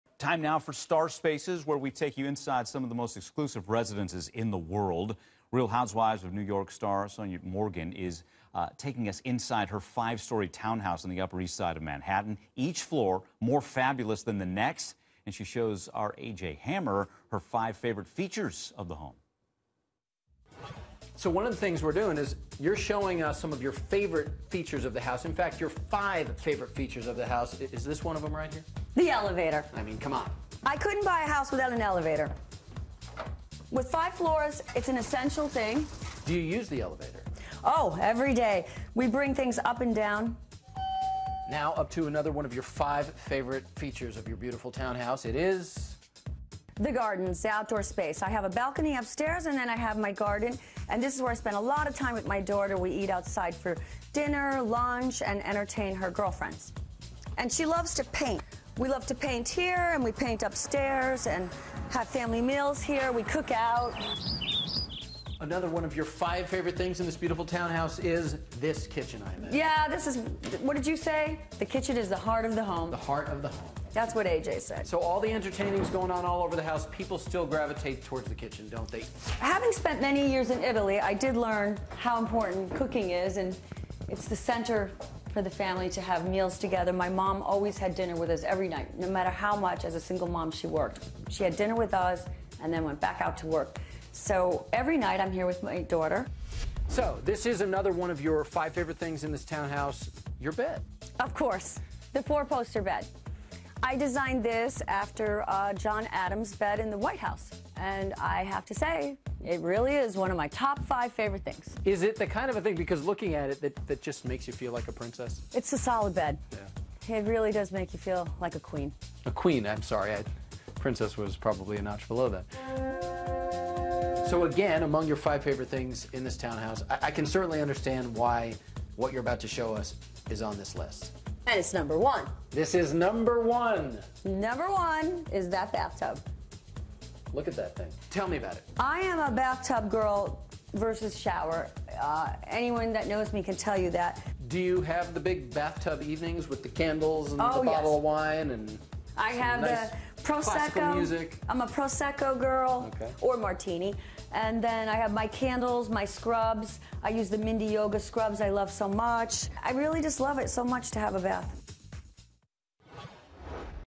Five best parts of 'RHONY' Sonja Morgan's home
HLN goes inside the home of "Real Housewives of New York City" star Sonja Morgan. Morgan's Upper East Side townhouse can often be seen on her Bravo reality show, but A.J. Hammer got an exclusive tour of her five-story home and the reality star points out her five favorite features.From the outdoor garden to the sprawling kitchen, check out Morgan's fabulous star space.